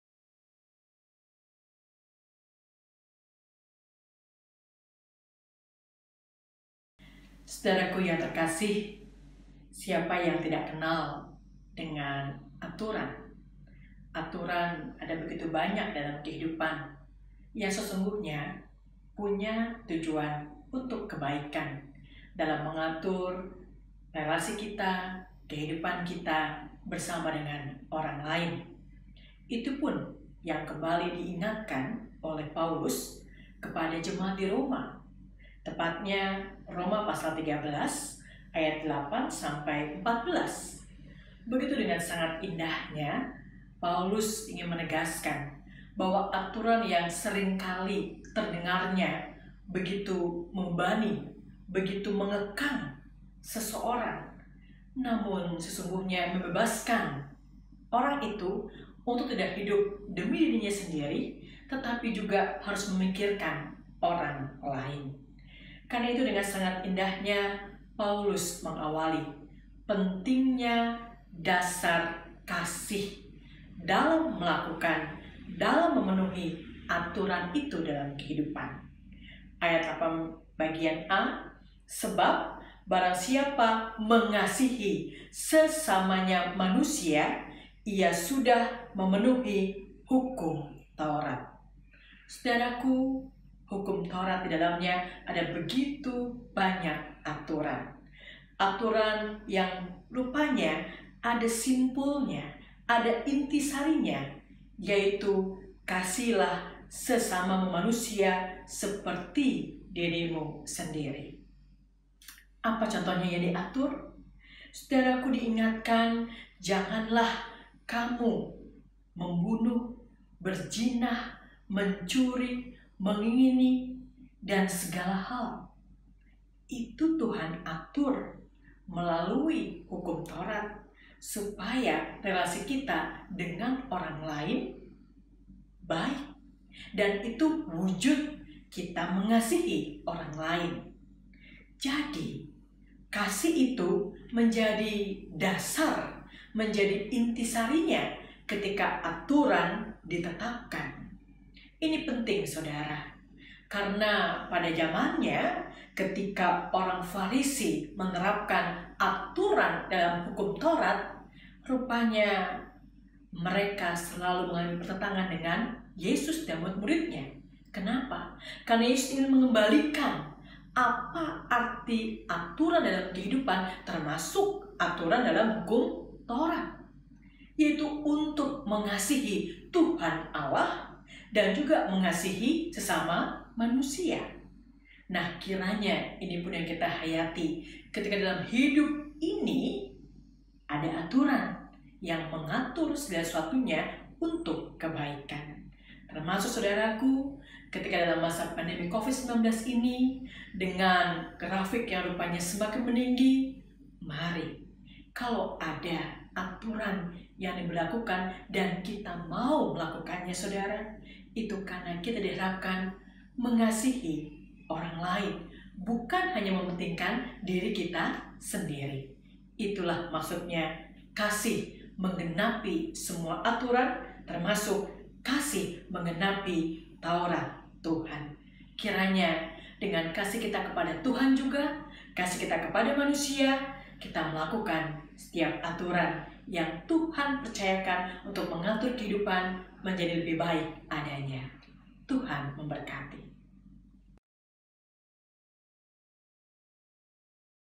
SAPAAN ROHANI, SENIN 20 Juli 2020 #86
SAPAANROHANI#86
hari ini oleh Pdt. Woro Indyas Tobing.
ATURAN HIDUP --- Roma 13:8-14

Sapaan Rohani hari ini di dalam hidup ini ada aturan yang mengatur segala sesuatunya untuk kebaikan. Bukan untuk kepentingan sendiri namun untuk mengasihi sesama. Dan Kasih mengatasi semua aturan itu yaitu mengasihi TUHAN Allah dan juga mengasihi sesama manusia. Amin.